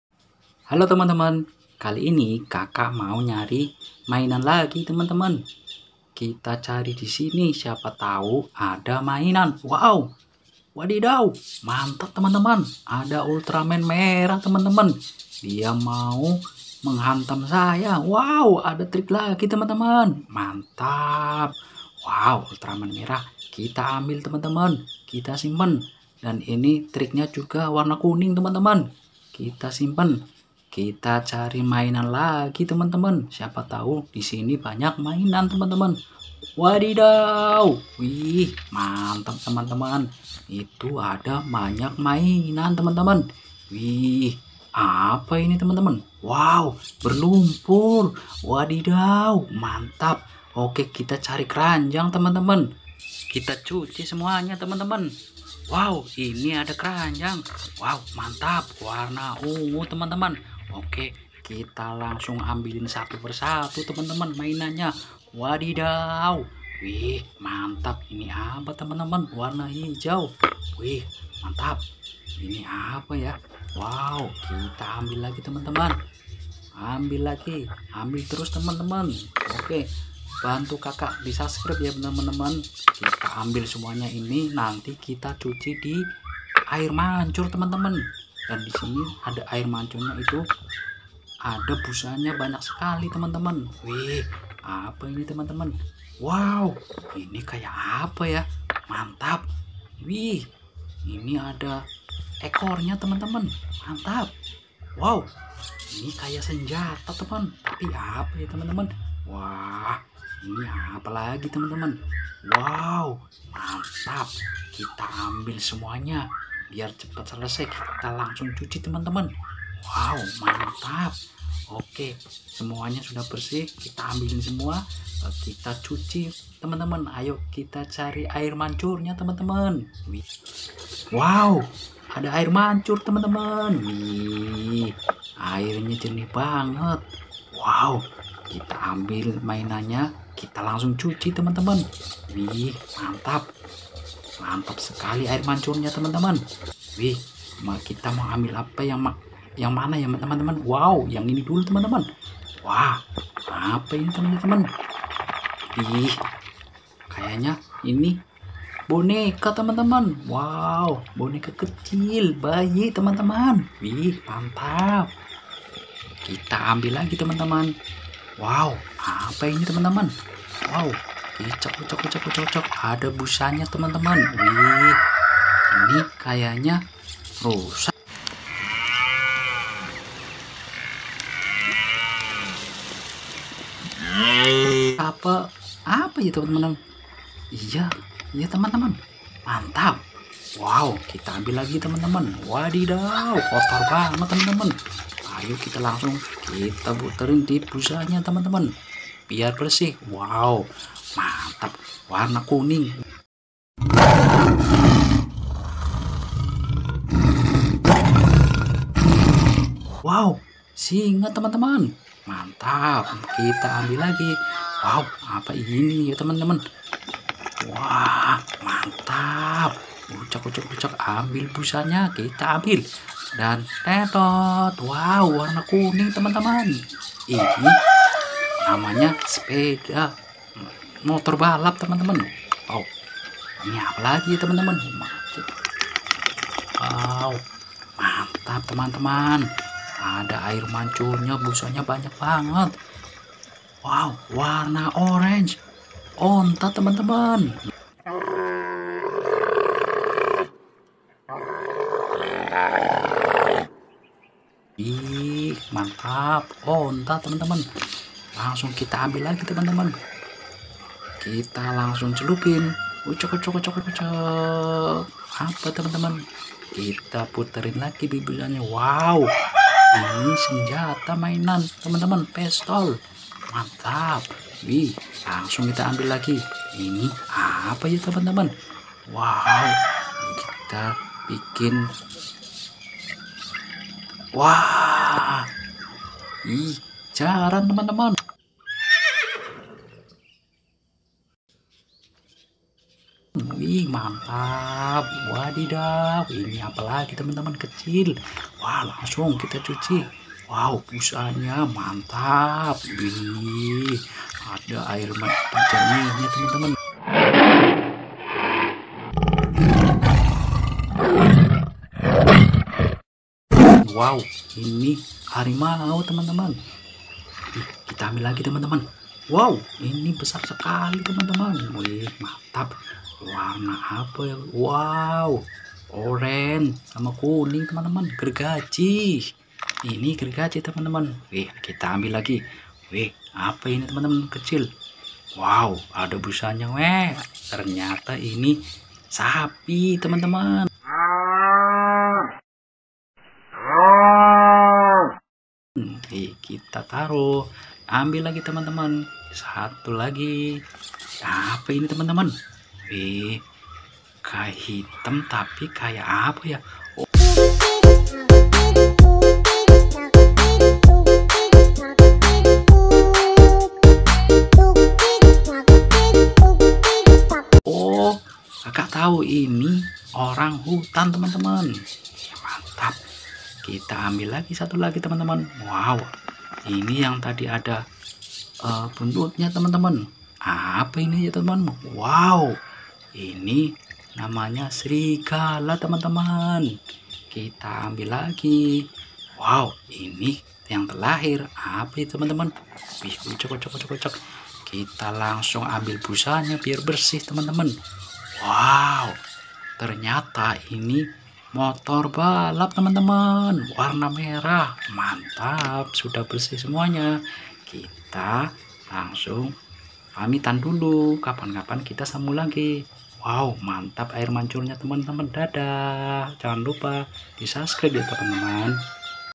MEMBERSIHKAN MAINAN DI AIR MANCUR
harimau,singa,jerapah,onta,rusa,kuda,sapi,zebra gajak,buaya,bebek dan mainan lainya

jangan lupa dukung channel mainan oyeeng ia teman teman, dengan cara SUBSCREB